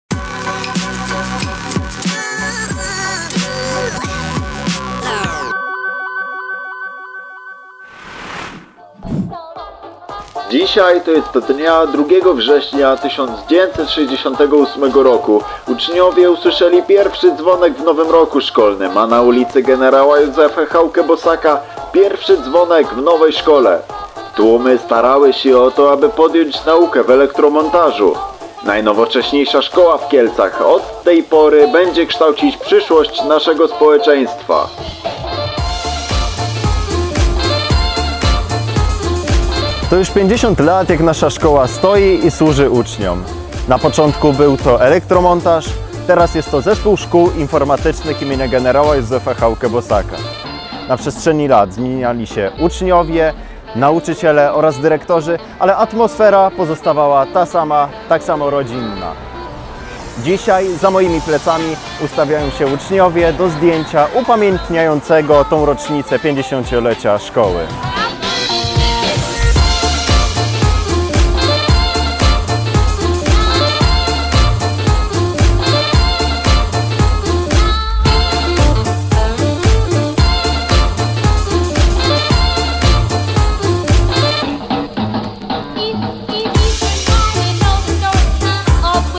50-lecie ZSI -zdjęcie pamiątkowe
Z okazji  50- lecia naszej Szkoły z inicjatywy Samorządu Uczniowskiego zostało wykonane grupowe zdjęcie upamiętniające ten, jakże piękny jubileusz. Ciężko było zmieścić w kadrze wszystkich, którzy chcieli wziąć udział w akcji, ale mamy nadzieję, że w większości udało się odnaleźć siebie na tej pamiątkowej fotografii.